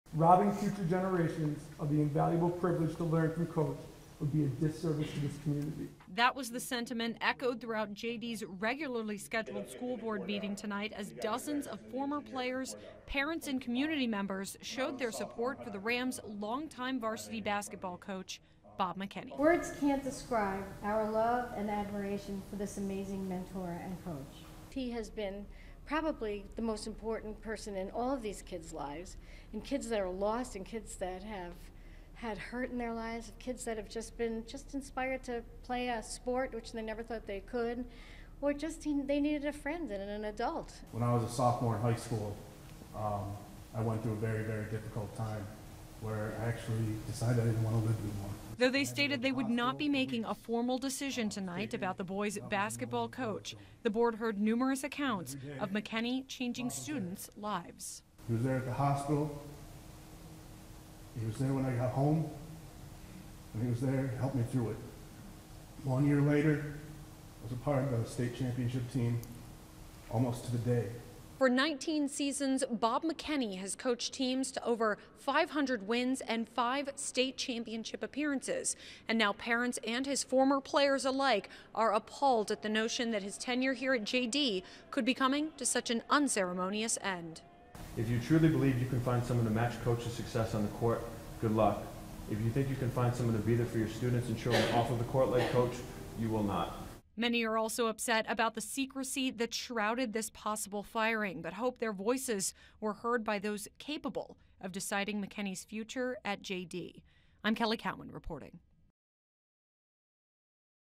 Former JD students, parents come out in full support of Bob McKenney
Parents and former players are appalled by the secrecy surrounding JD varsity basketball coach Bob McKenney's possible termination.